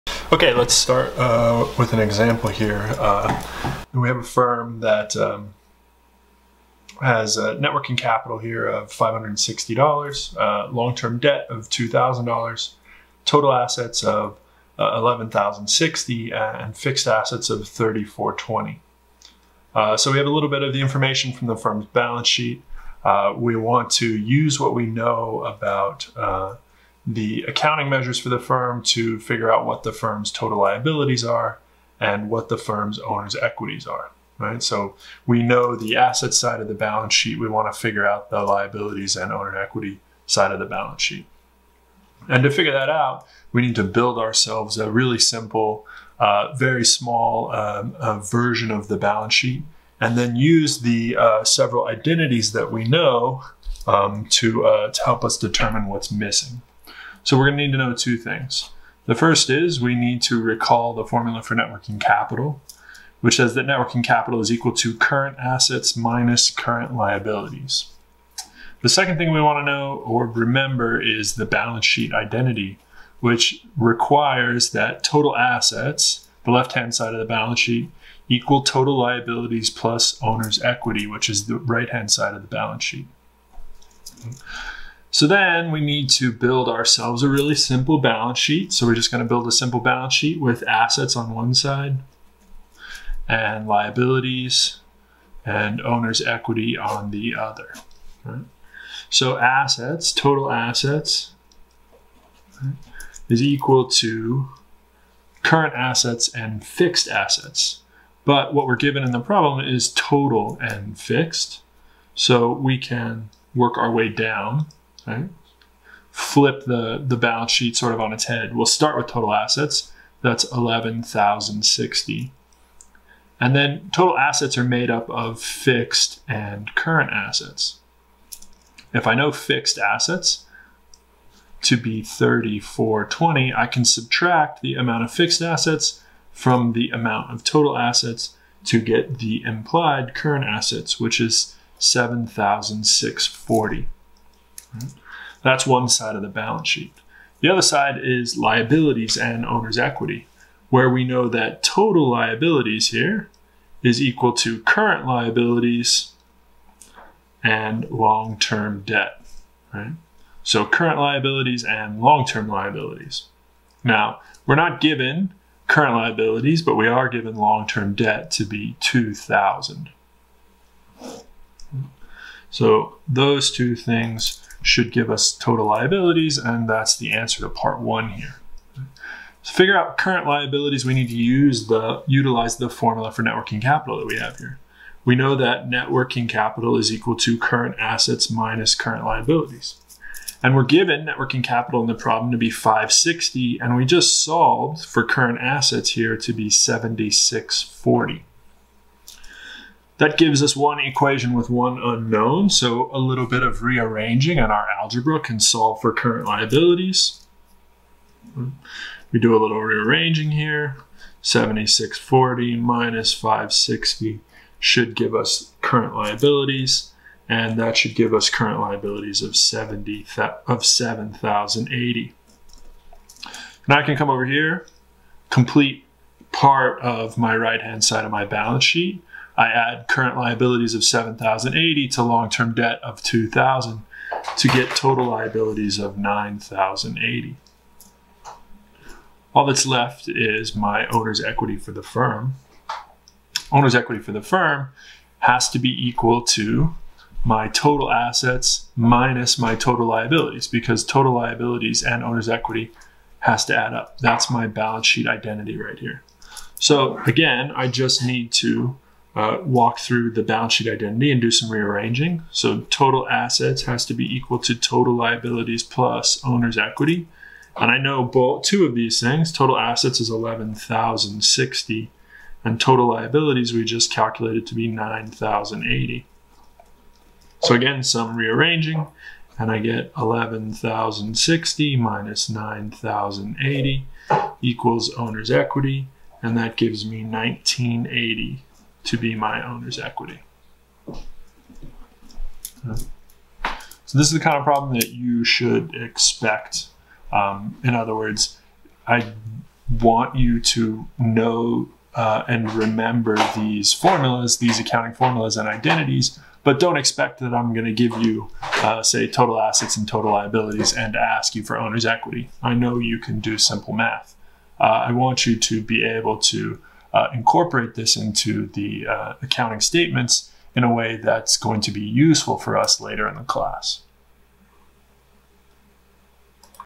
Chapter 2 Examples - NWC
This is an example of using a firms net working capital and your knowledge of the balance sheet identity to complete a simple balance sheet for an unknown firm.